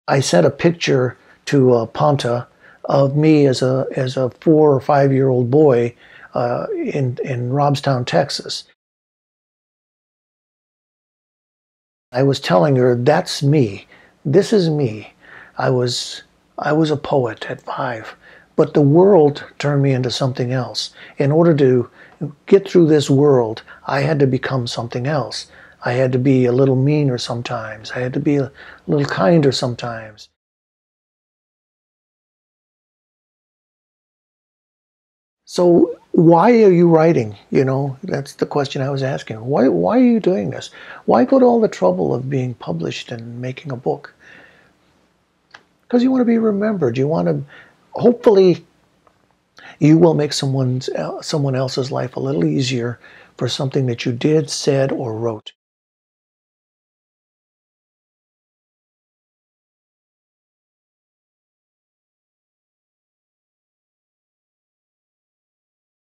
Coming soon to Passager Presents: Gilbert Arzola, author of Prayers of Little Consequence
Do you remember what you wanted to be when you grew up? Passager writer Gilbert Arzola explains that he was a poet at 5 but the world changed him. Full interview coming soon. From "Of Some Renown," a video series. Spring 2020. Videography by Gavin Rivera.